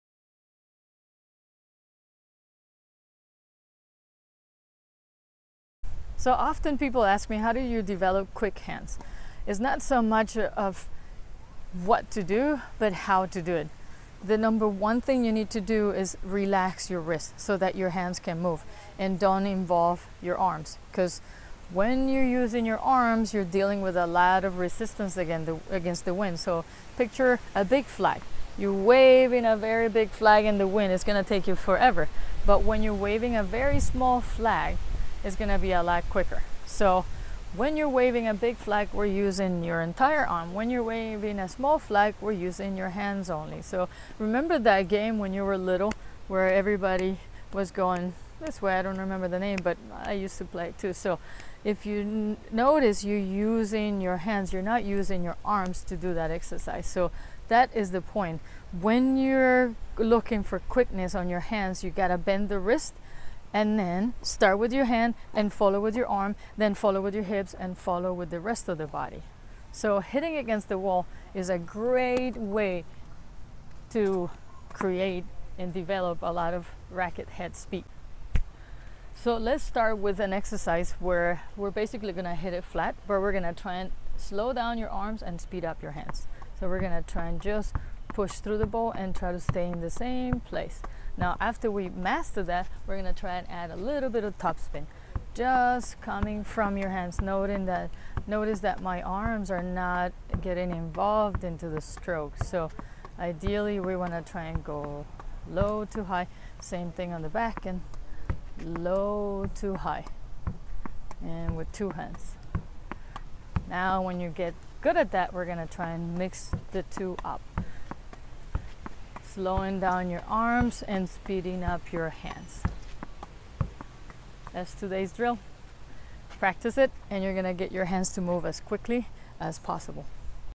How to develop quick hands
Loosen up your wrists to develop quickness in your hands.